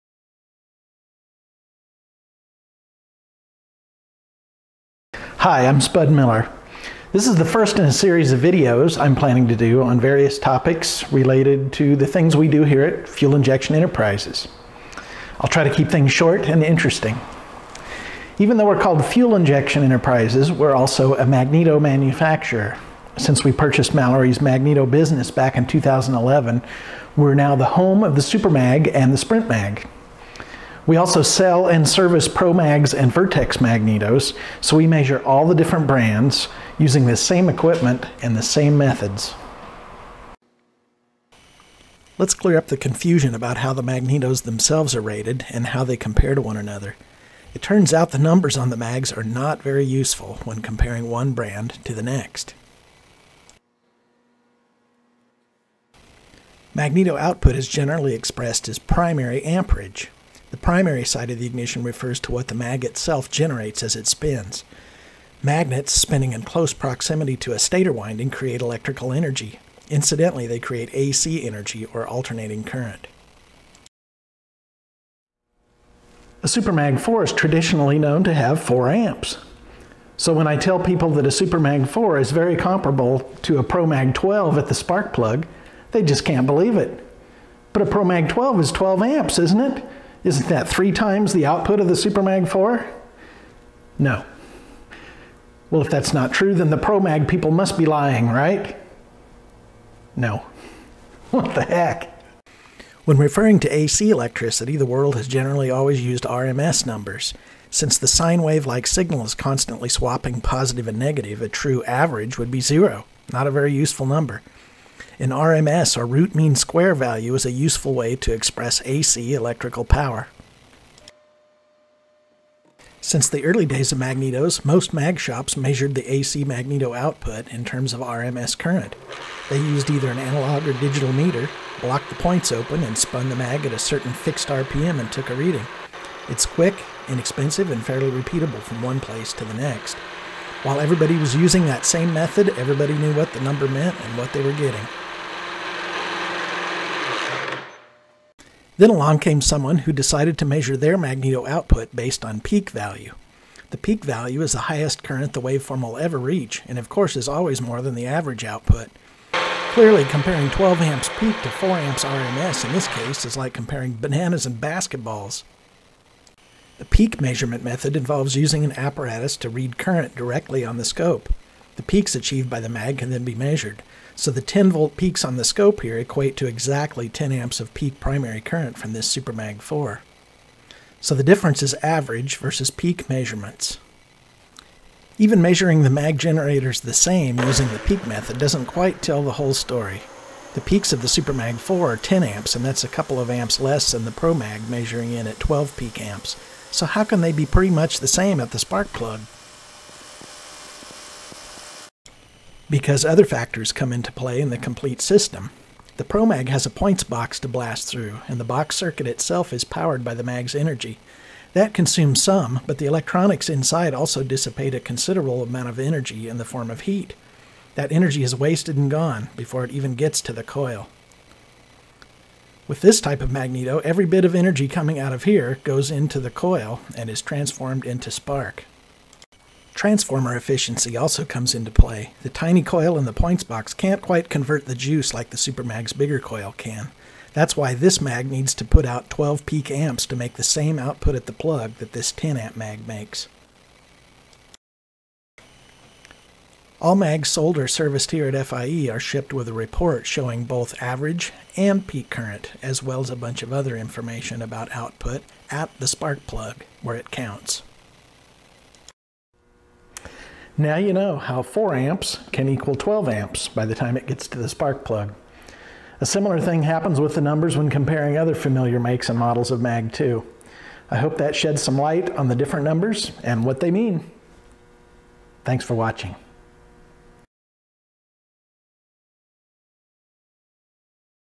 Measuring and Comparing Magneto Generators - Spud Miller, FIE
Spud Miller from FIE demonstrates measuring magneto primary output two ways and what the numbers on the labels mean. How does a Super-Mag and Pro-Mag really compare when measured using the SAME method?